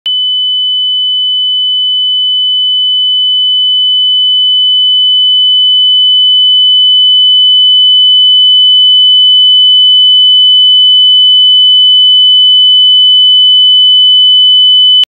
Howler / Off-the-hook tone (Bangladesh and more). Call-progress tones. Phone sounds. Sound effects.
CALL-PROGRESS TONES: HOWLER (Bangladesh and more)

HOWLER TONE: (also off-hook warning, off-hook tone, or howler tone) is a telephony signal for alerting a user that the telephone has been left off-hook without use for an extended period, effectively disabling the telephone line.

Frequency in Hz: 3000.
Cadence in seconds: continuous, graduated level.

Other countries that adopted this tone: French Polynesia, Seychelles, Vanuatu.

Source: Annex to ITU Operational Bulletin No. 955 – 1. V.2010.